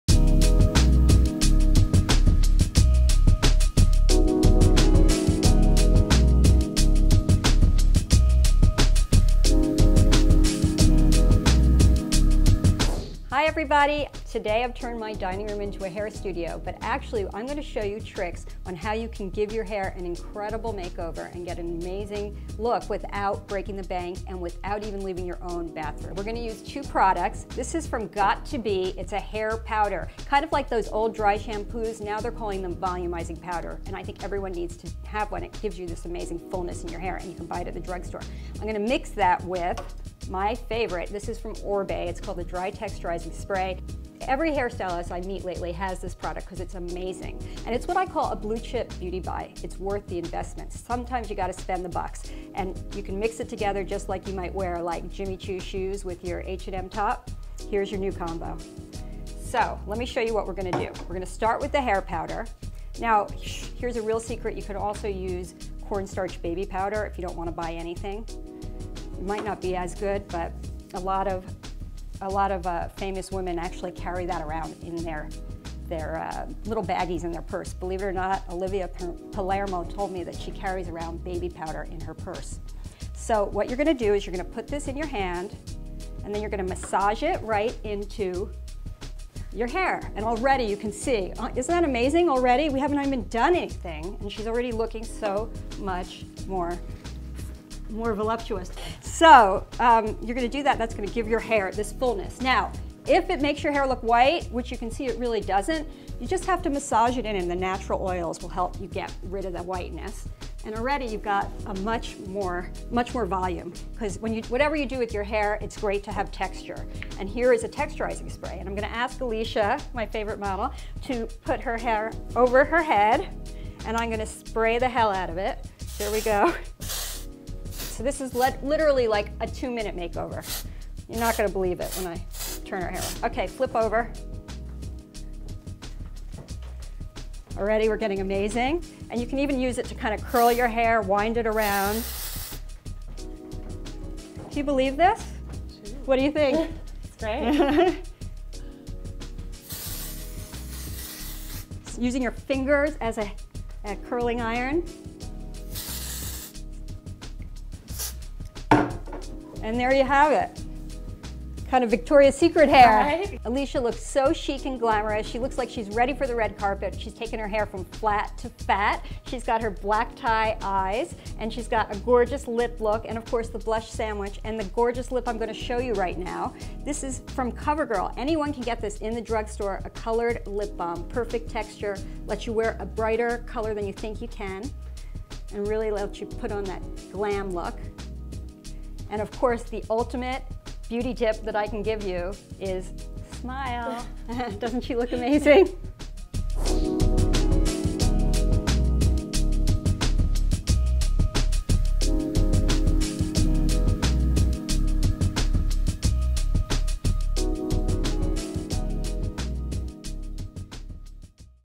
10 Second Hair Makeover by Andrea Pomerantz Lustig
How to use dry texturizing spray to get instant volume.